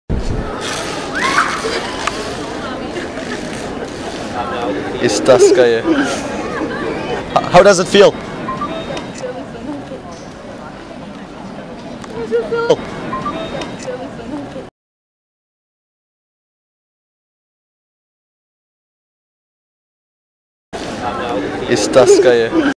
Hooters girls prank
Hooter Hooters girls prank. Wrapping hooters girls in public with tape . So they can nopt move anymore ! Must see ! This video was compiled from video(s) from